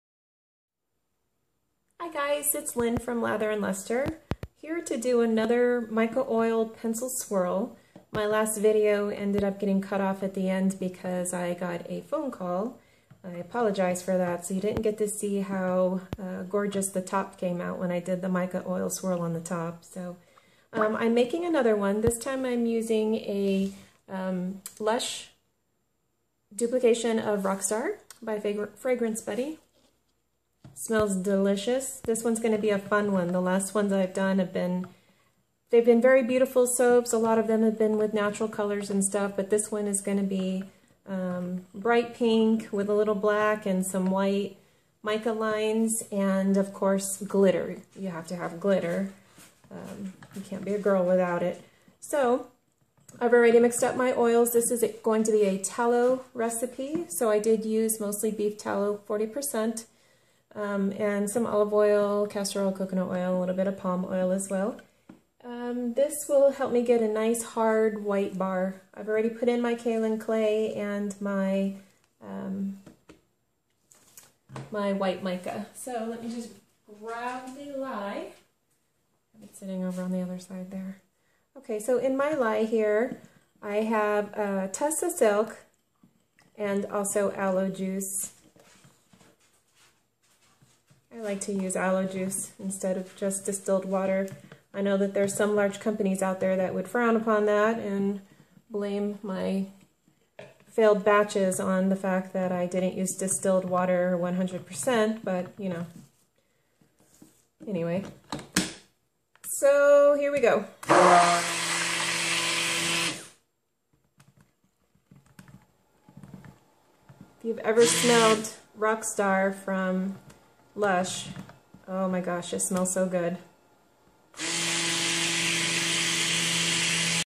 Mica Oil Pencil Swirl - "Rock On" part one
A mica oil pencil swirl using the dupe of "Rockstar" by Lush. This is in 2 parts due to space issues.